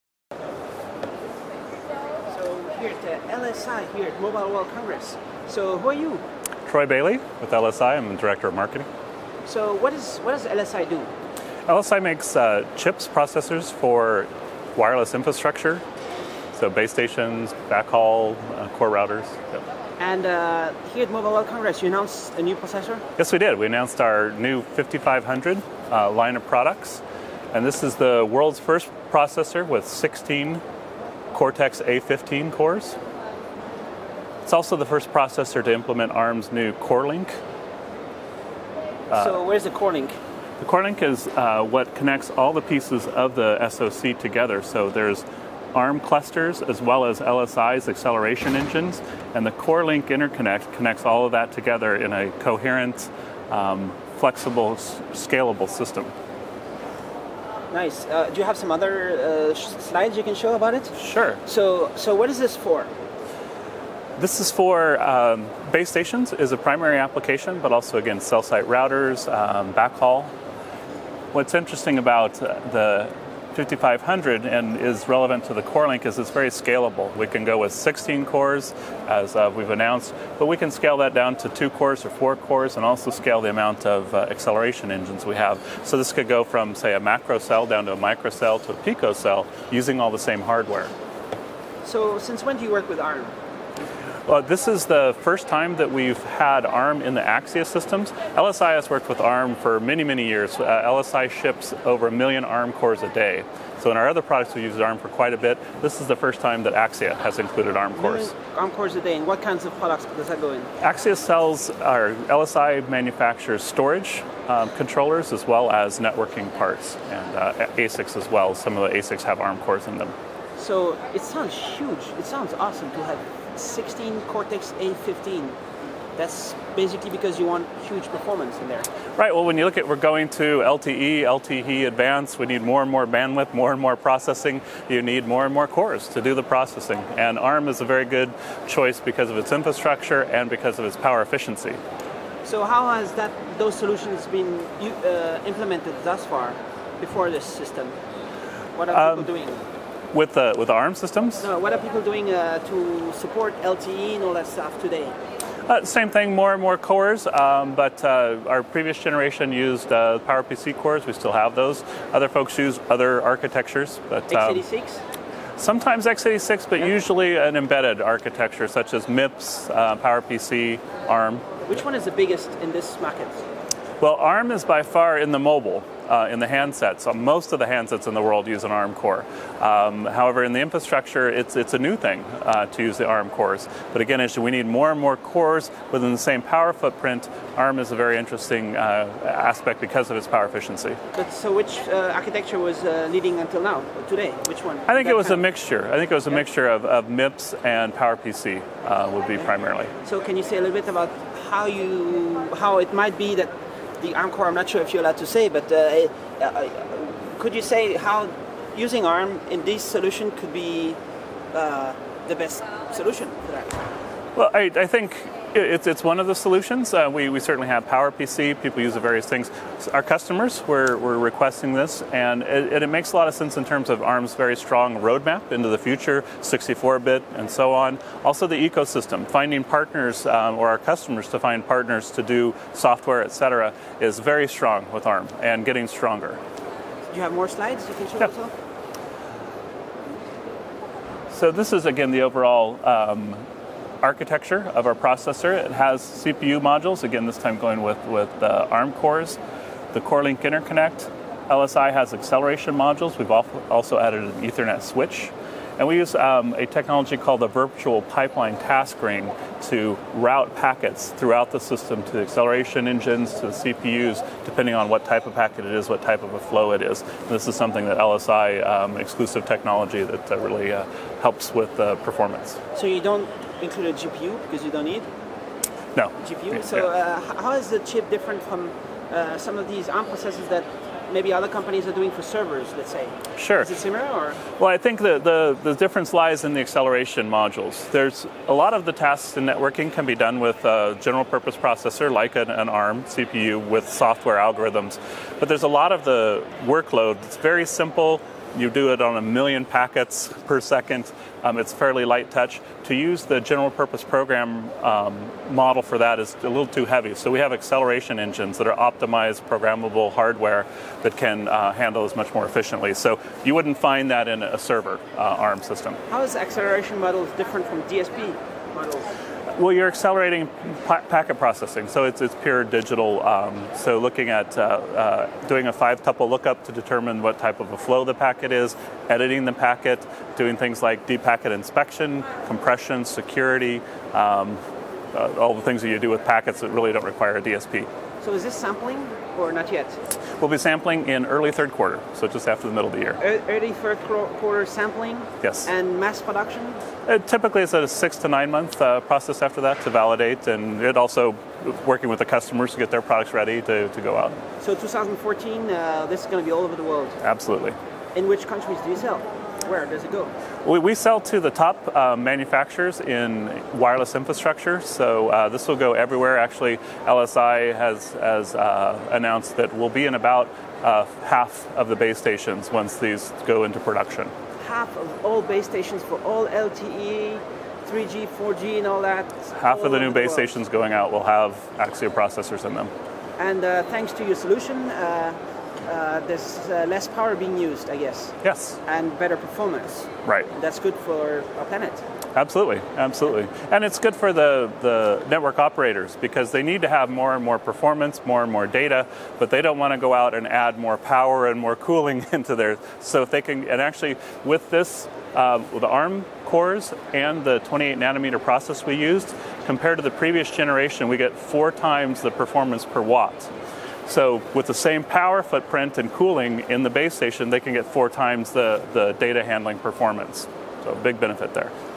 LSI Axxia 5500 announced, 16-core ARM Cortex-A15 for network infrastructure
LSI designs semiconductors and software that accelerate storage and networking in datacenters and mobile networks. At Mobile World Congress 2013, LSI is introducing the Axxia 5500 16-core ARM Cortex-A15 to provide scalability, performance and low power consumption to meet the growing demand for mobile broadband.

Filmed at Mobile World Congress 2013 in Barcelona using the Panasonic GH3 camcorder with 12-35mm lense on a Tiffen Steadicam Merlin 2 with vest